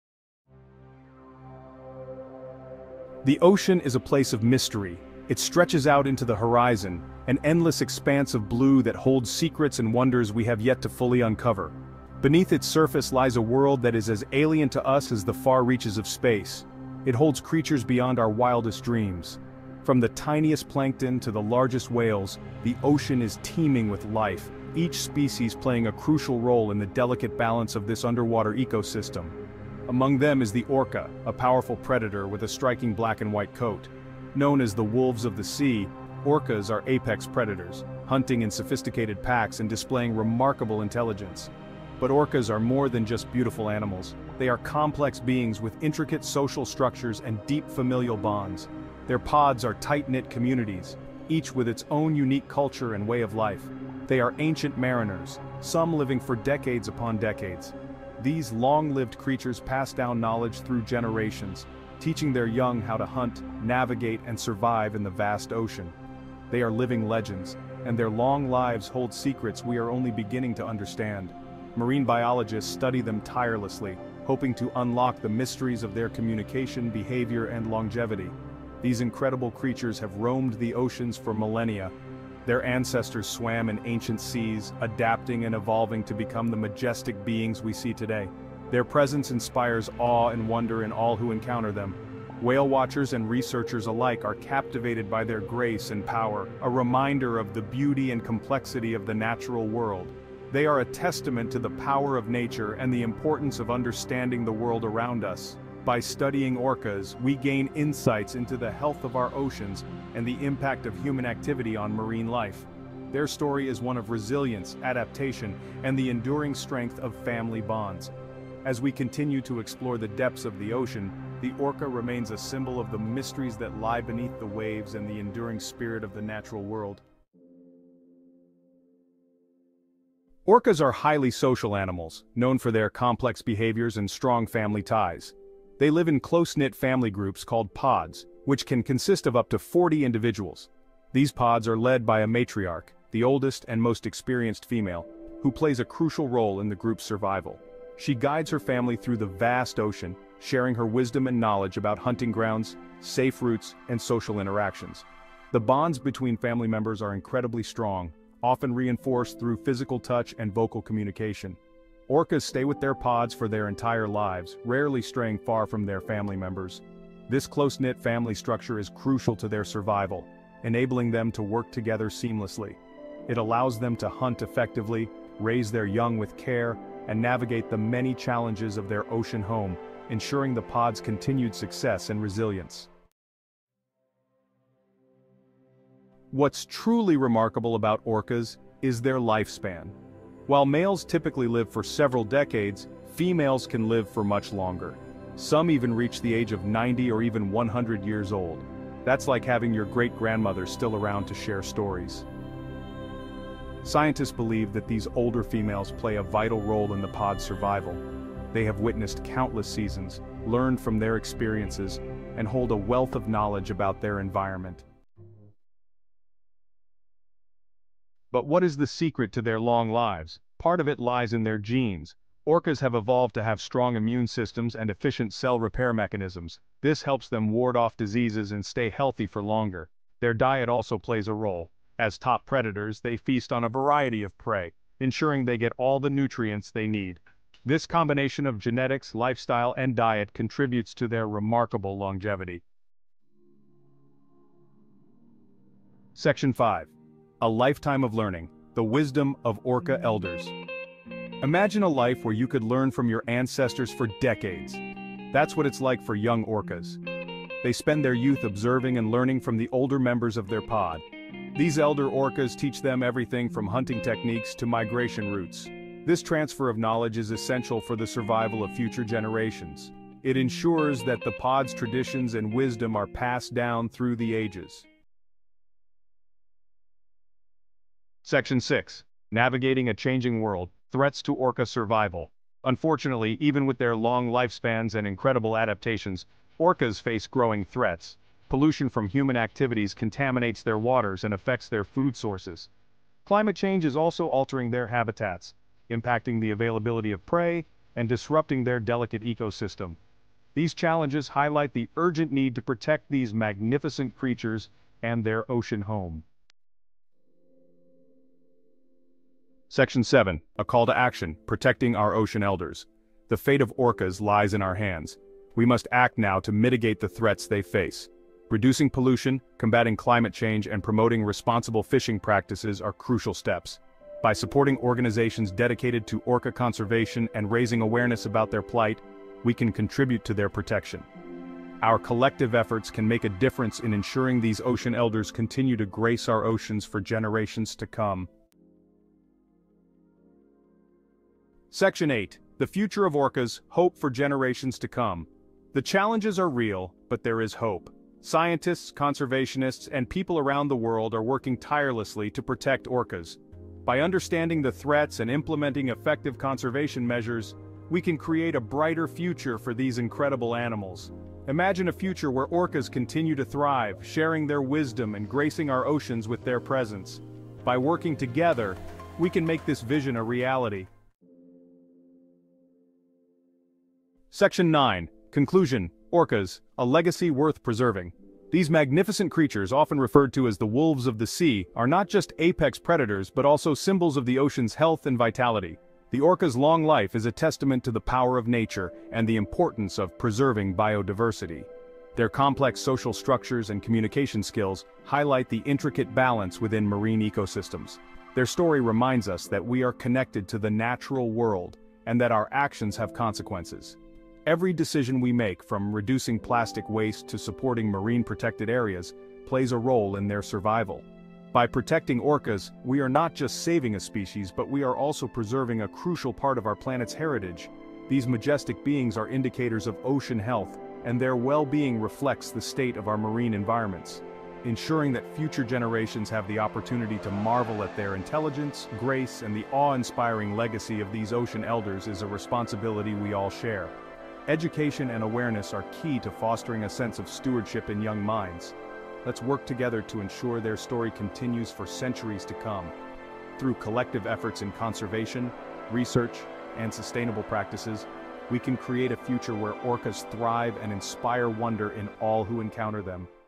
Ancient Orcas: The Fountain of Youth in the Ocean
Discover the majestic world of orcas in our latest video! Dive into the lives of these powerful predators with their iconic black and white coats, and explore their ancient mariner legacy. Learn about the critical roles of matriarch-led pods, the strong family bonds, and the vital wisdom of elder females that contribute to their survival. Understand how their genetics, lifestyle, and diet enable their remarkable longevity.

We also discuss the urgent threats from pollution and climate change affecting orca habitats and the vital steps we need to take for their conservation. Join us in highlighting the importance of protecting these incredible creatures for future generations.

OUTLINE:

00:00:00 Ancient Mariners of the Deep
00:02:07 The Orca Pod
00:03:03 Orcas' Remarkable Lifespan
00:03:35 Genes and Immunity
00:04:02 The Wisdom of Orca Elders
00:04:35 Threats to Orca Survival
00:05:06 Protecting Our Ocean Elders
00:05:38 Hope for Generations to Come
00:06:11 Orcas – A Legacy Worth Preserving